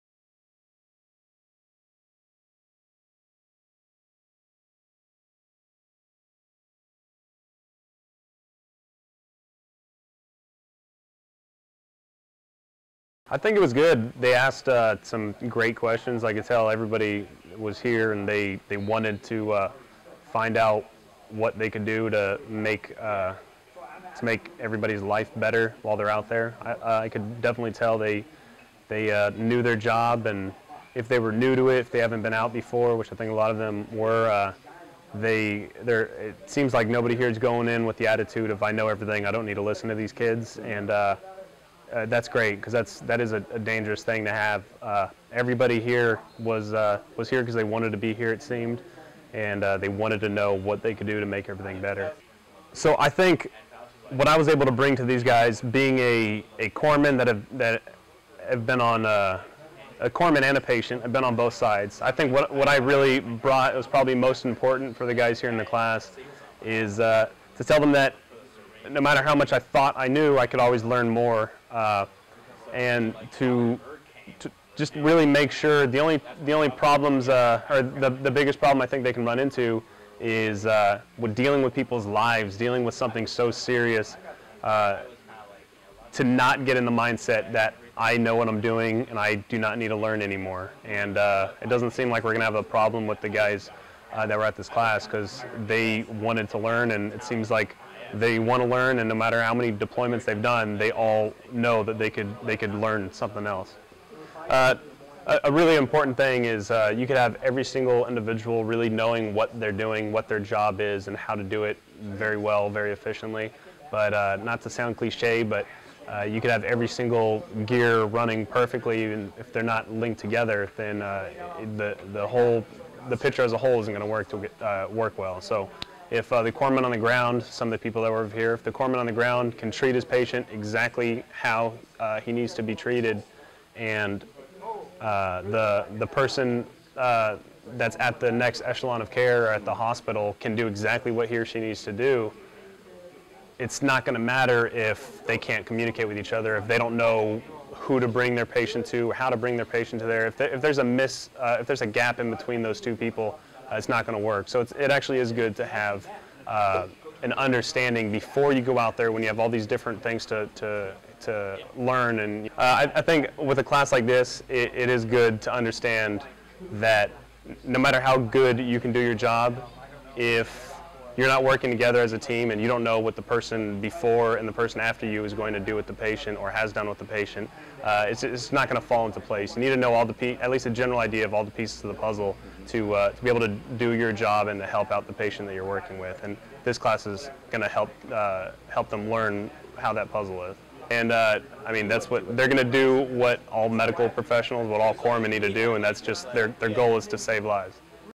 Hospital Corpsman 3rd Class Redmond Ramos
Hospital Corpsman 3rd Class Redmond Ramos, assigned to the Wounded Warrior West Battalion, talks about the importance of the NEMTI-run Role 3 Kandahar Course for medical personnel deploying to contingency areas. Ramos was injured by an IED blast, losing his left leg, during operations with Marines in Afghanistan.